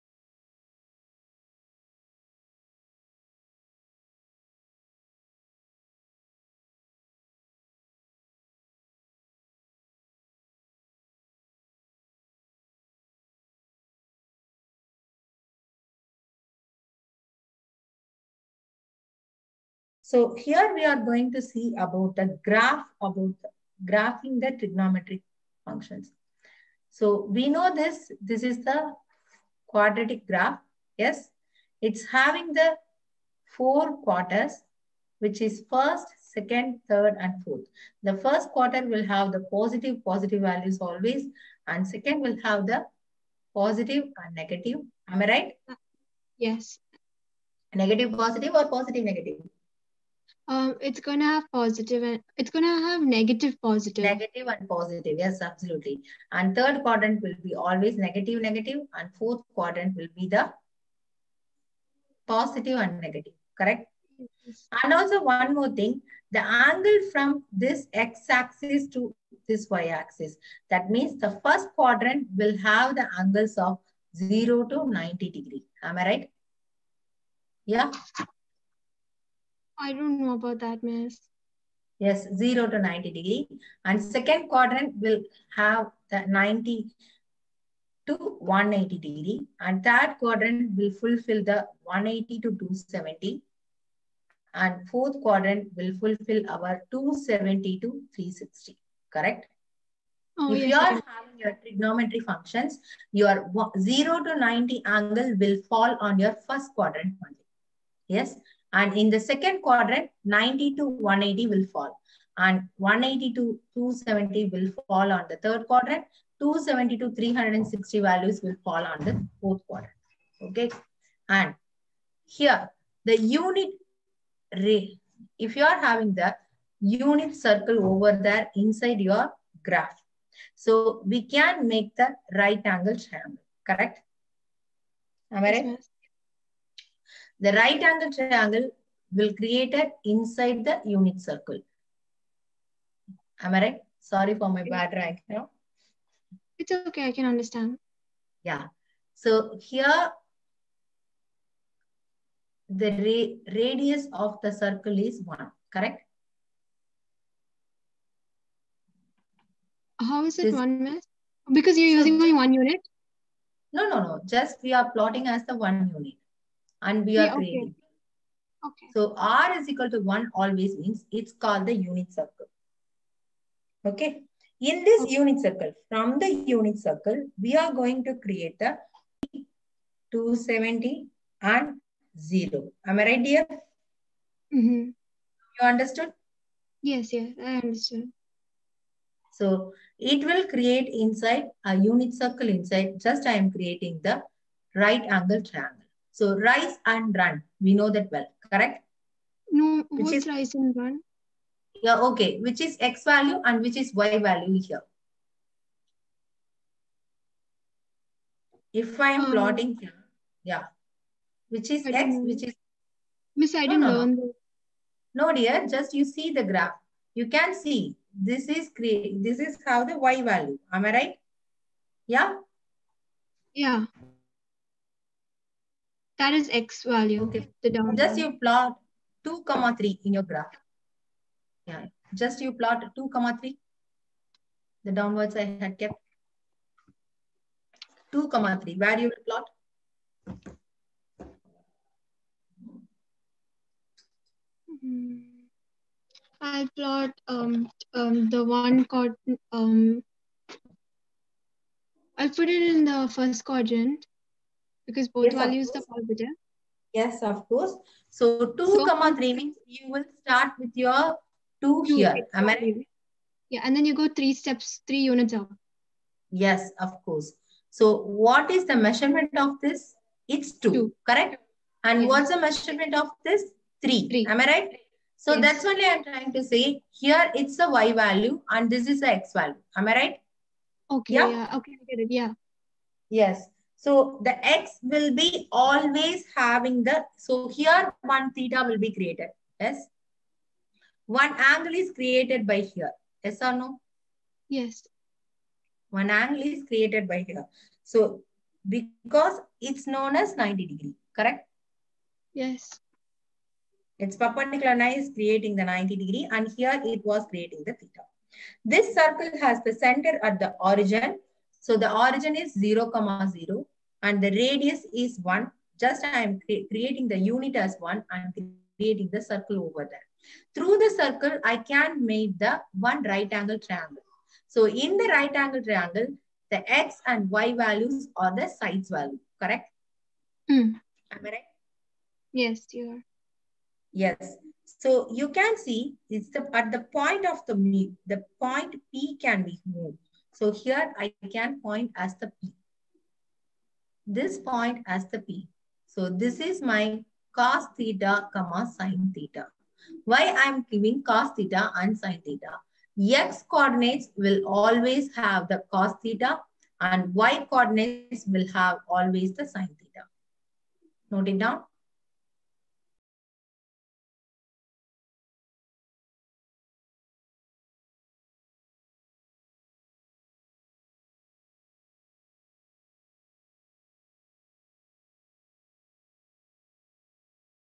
Graphing trigonometric functions | mathematics session | MQube tuitions
Hello all

The above video contains a small part of online Math session of Sheena (student) with MQube tuitions